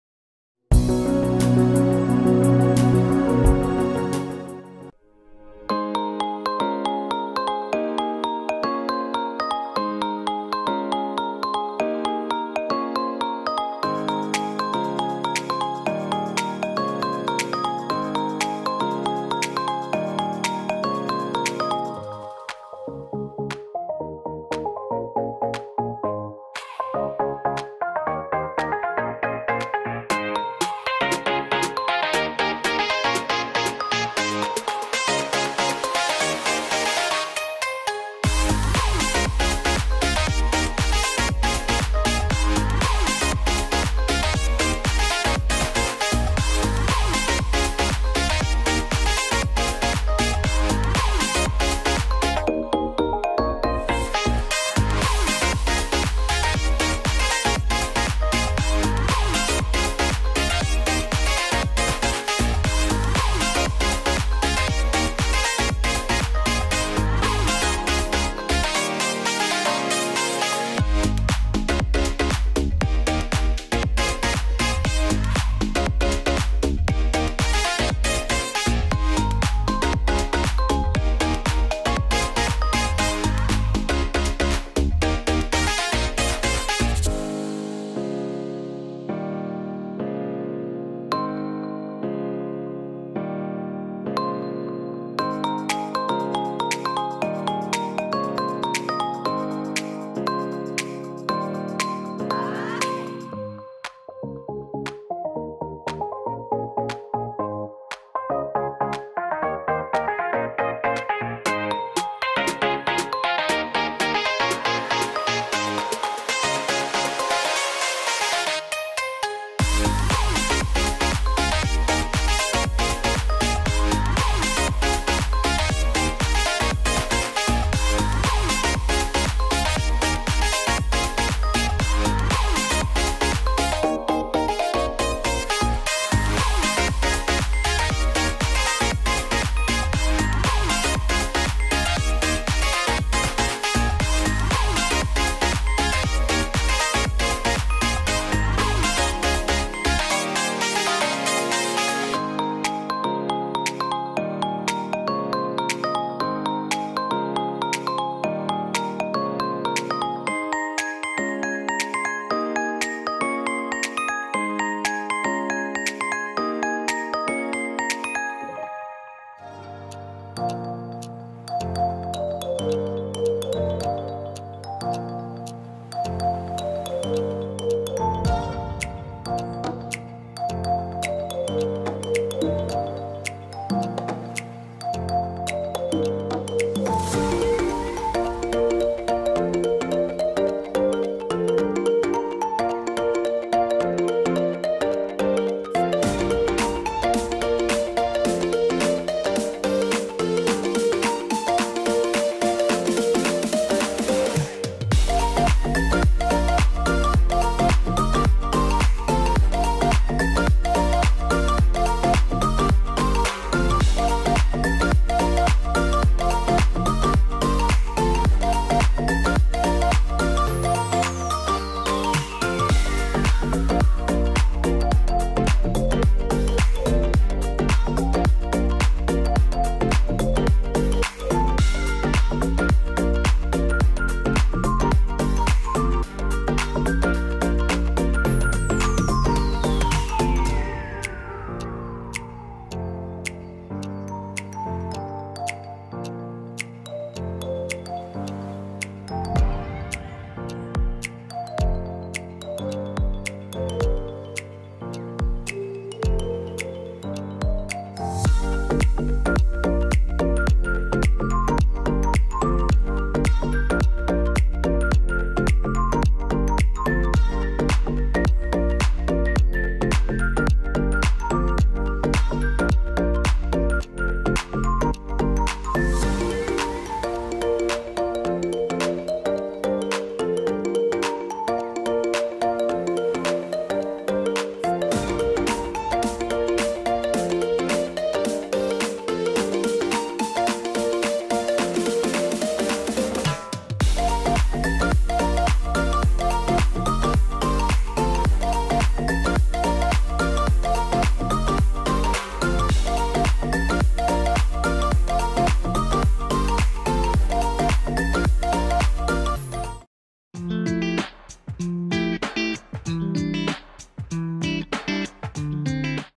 Pink Ping World 🌈 Toca Life World DIY / How to draw Rainbow Hair / Paper crafts
How to draw PPW Toca Life World
How to draw Rainbow Hair
•Sakura White Gelly Roll pen
•Touch Twin Markers 60
•Micron Pen Set
•Double-Sided Tape for Crafts
•Arrtx Skin Tone Markers
•Water color pencils